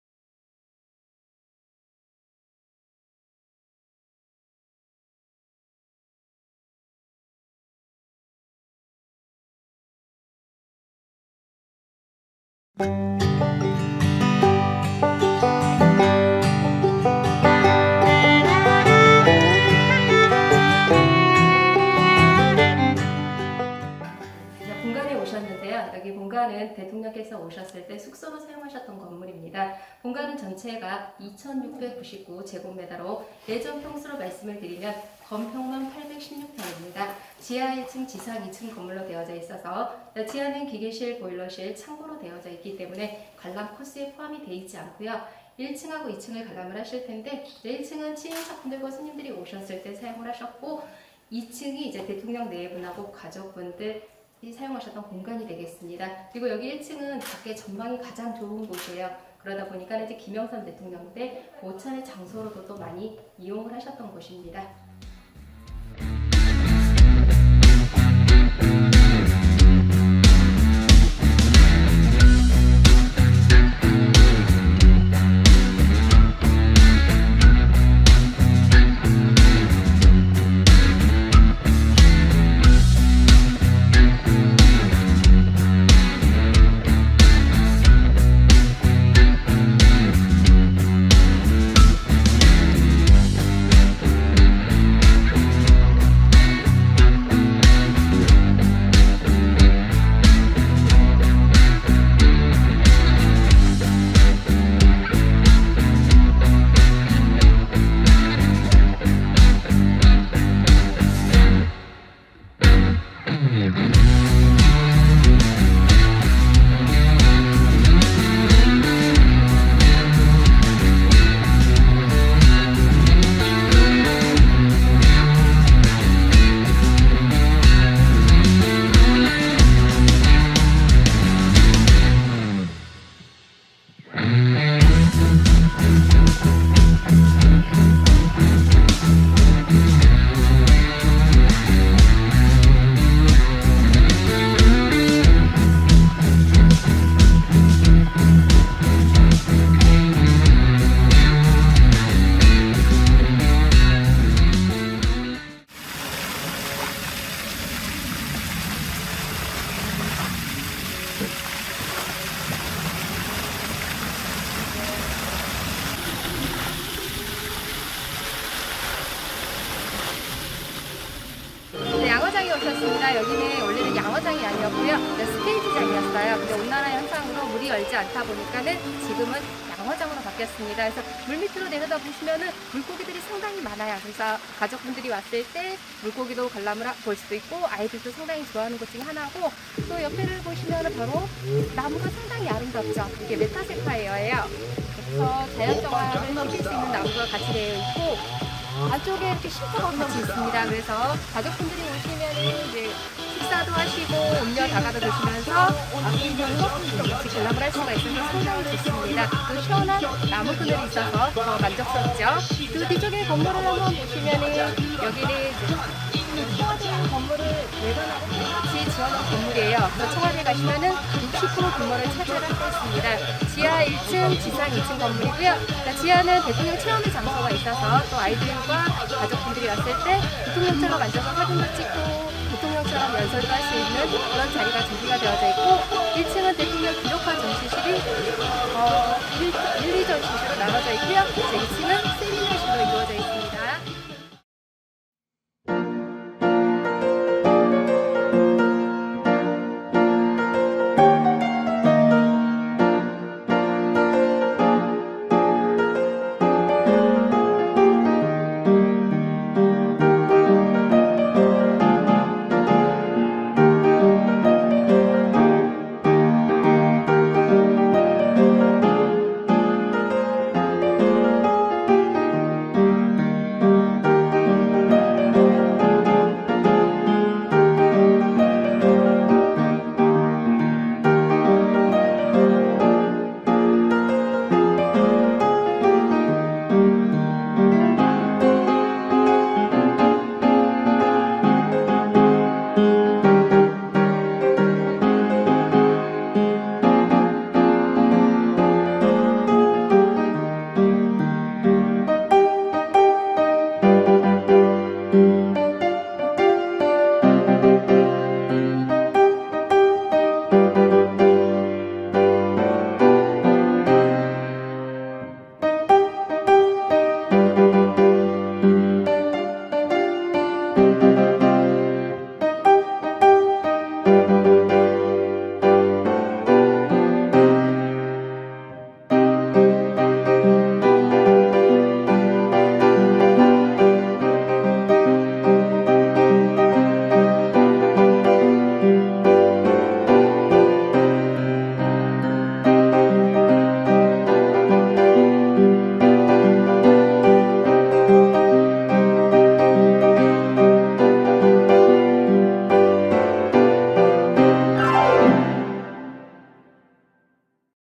대통령 별장 '청남대'의 봄
한때는 대통령의 별장이었던,
대청호반에 자리 잡고 있는 청남대를 찾아가는 길목마다
봄 꽃들의 향연이 시작되고 있다.
올해로 8회째를 맞는 청남대 봄꽃축제 ‘영춘제’가
“봄을 봄, 희망을 봄”이란 주제로
오는 4월 24일(토)부터 5월 9일(일)까지 청남대에서 열린다.
월요일을 제외한 14일간 야생화, 수목분재 전시와
목,석부작, 솟대, 현대서각 전시회도 열린다.